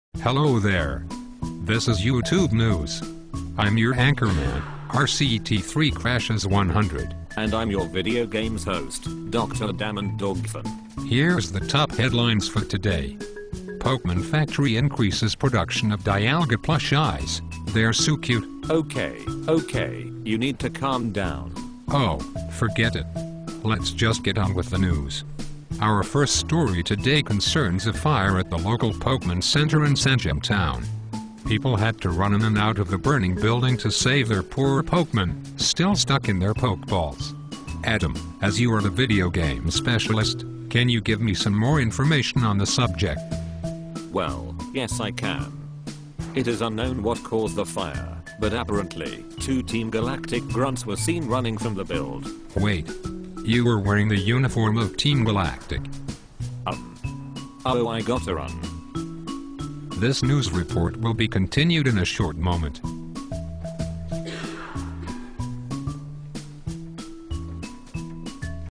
Youtube News: Episode 1 - Pokemon Center
RCT3Crashes100 and Adam start their first news show!